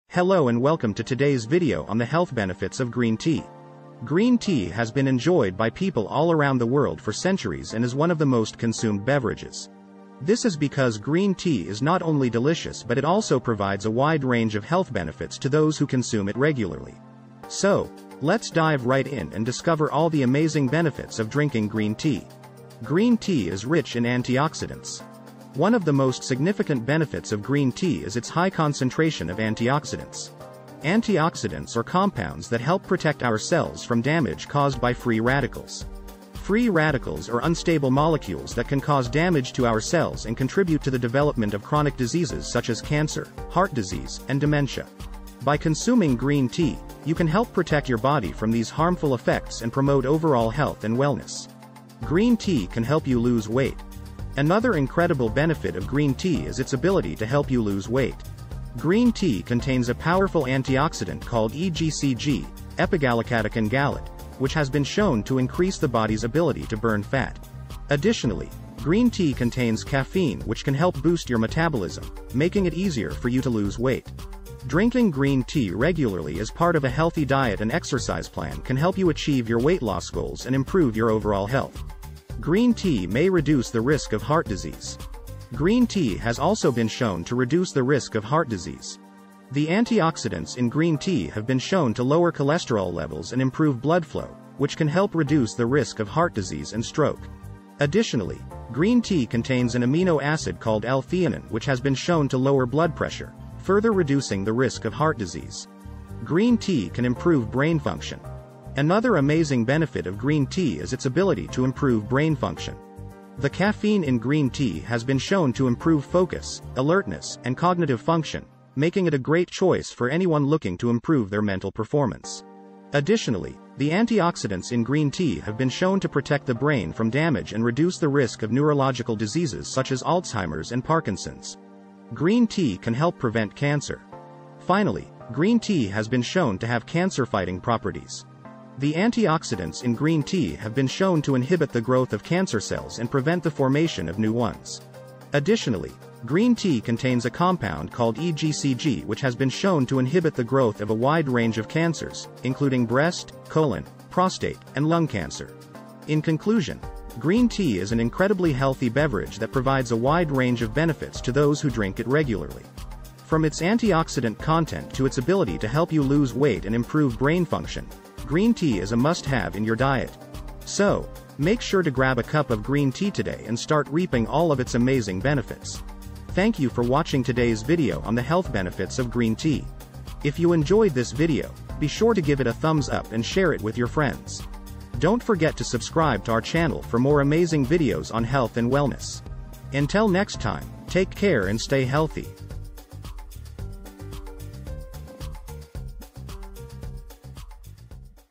The Top 5 Reasons to Drink Green Tea Every Day
Unleash the power of green tea for your health and wellness! From boosting brain function to improving heart health, learn the top 5 benefits of green tea that you need to know. Drink to your health!